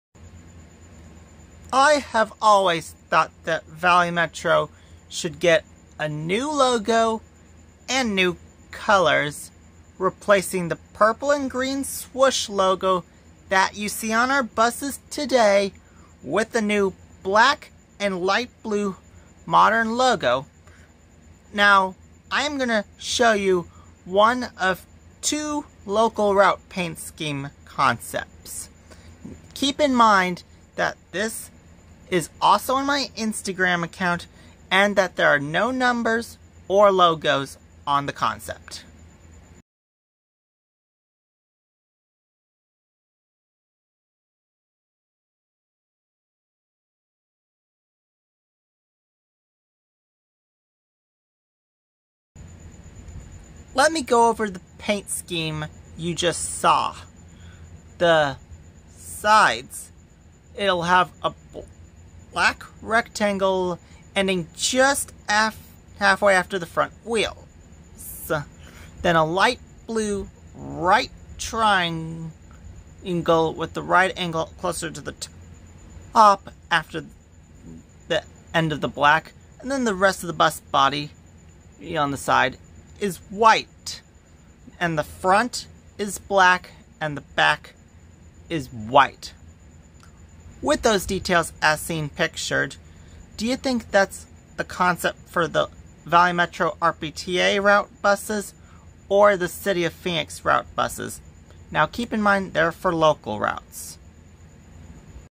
Valley Metro Scheme Concept Redo
I have always thought the Valley Metro should get a new logo and new colors... replacing the purple and green swoosh you see on our buses today with a more-modern black and light blue logo.

In this thought, I showed a concept of how one of two local route paint schemes:

*Sides feature a black rectangle til a little more halfway the front wheel, then a light blue right triangle whe 90 degree (right) angle on the top having the last triangle tip just before the end of the second window, with white to follow to the back.
*Black front panel.
*White back panel.

While you don't see numbers or logos on the paint scheme concept, do you think this would be the local route concept for the Valley Metro RPTA or the City of Phoenix?

Bonus thought: How do you think the other side's concept would look like?